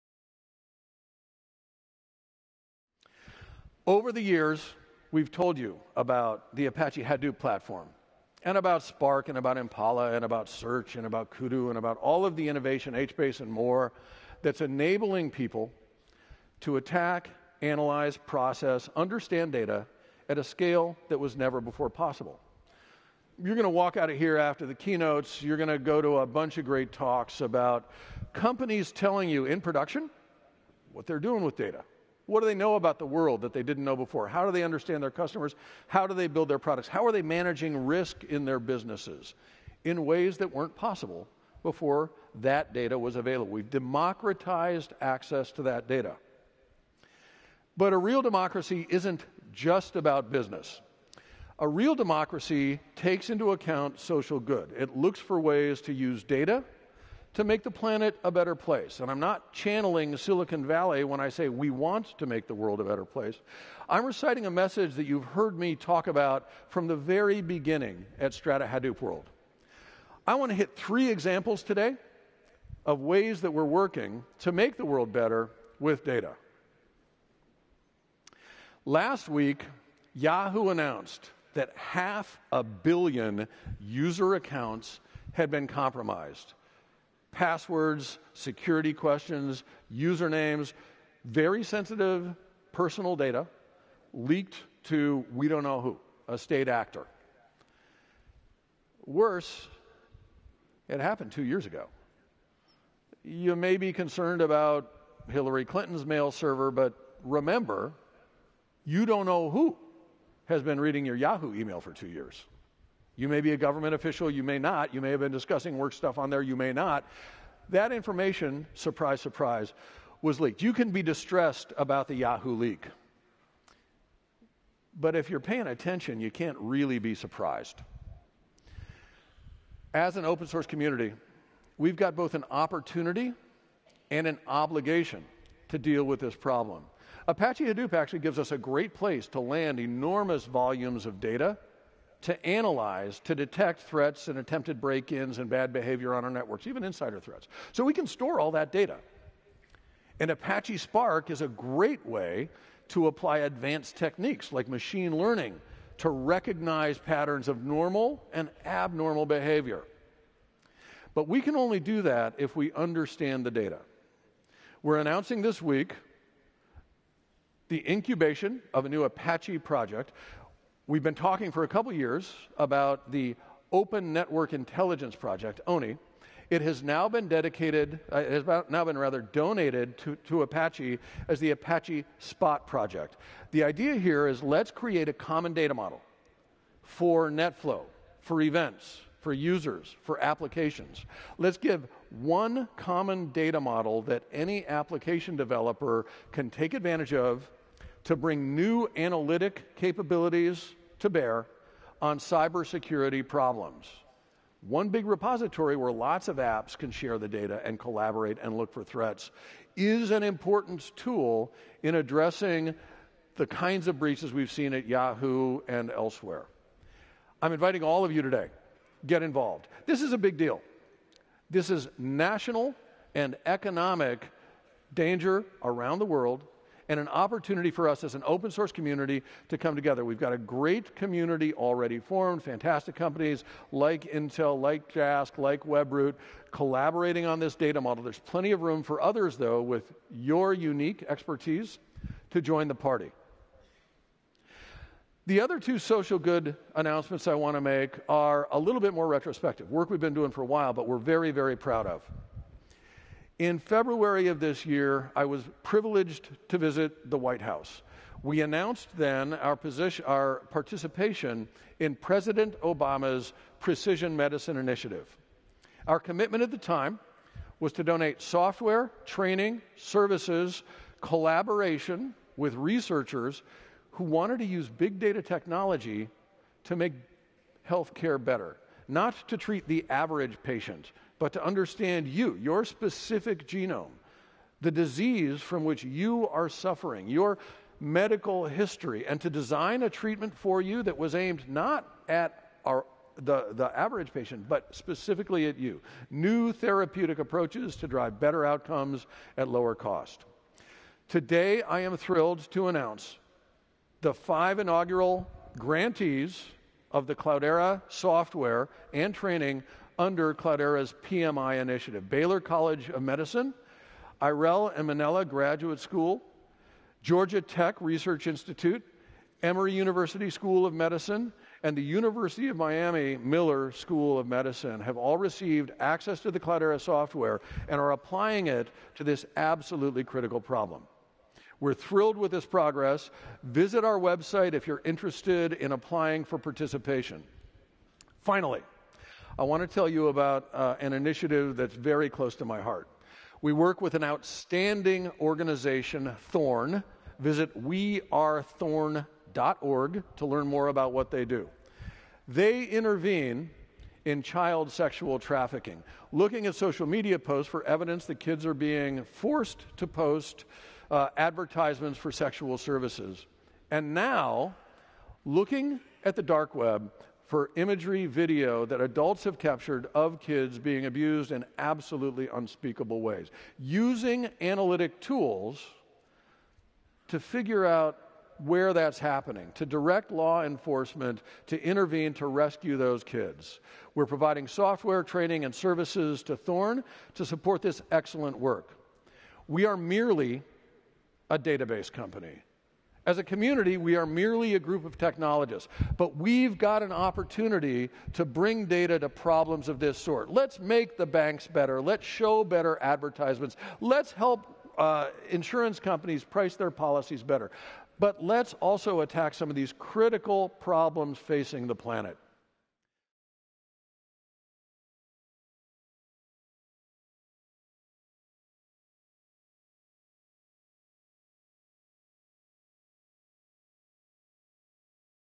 The new dynamics of big data - Mike Olson (Cloudera)
Since its inception, big data solutions have best been known for their ability to master the complexity of the volume, variety, and velocity of data. But as we enter the era of data democratization, there’s a new set of concerns to consider. Mike Olson discusses the new dynamics of big data and how a renewed approach focused on where, who, and why can lead to cutting-edge solutions.